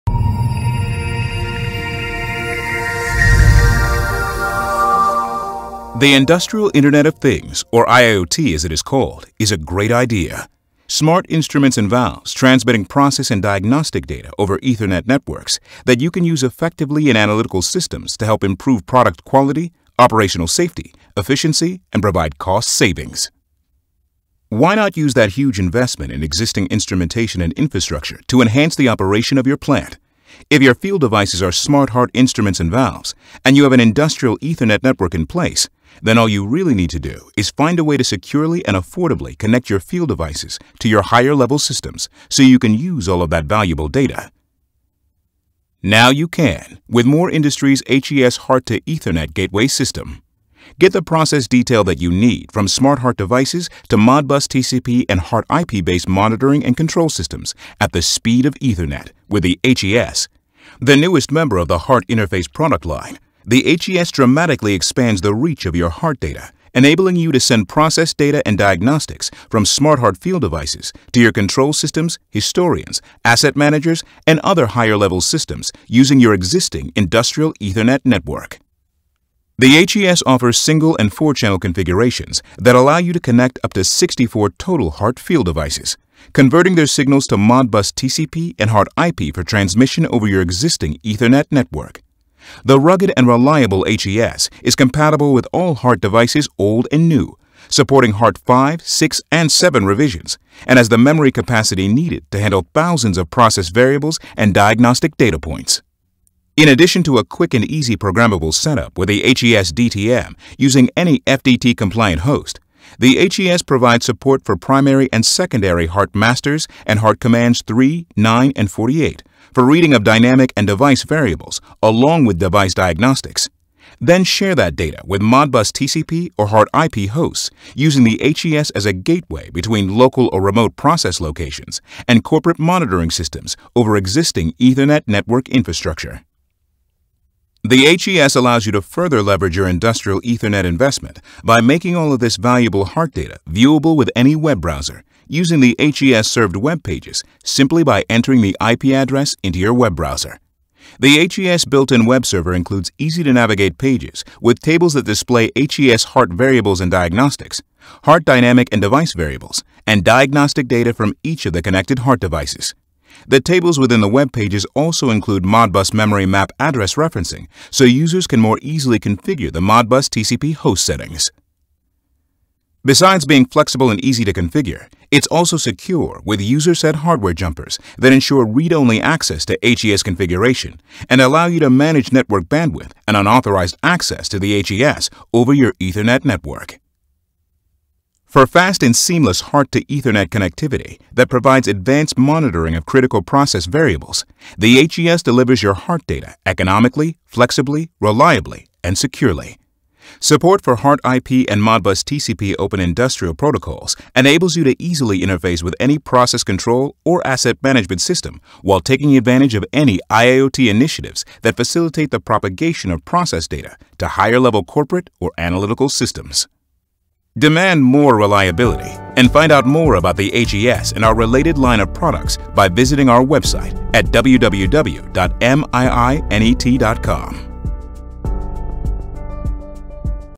The HES HART to Ethernet Gateway System Accelerates Your HART Data at the Speed of Ethernet
Accelerate your HART data. Timely knowledge about your process enables better decisions and faster preventive action. Now you can get the process detail that you need from your Smart HART devices to MODBUS/TCP and HART-IP based monitoring and control systems at the speed of Ethernet with the HES HART to Ethernet Gateway System from Moore Industries.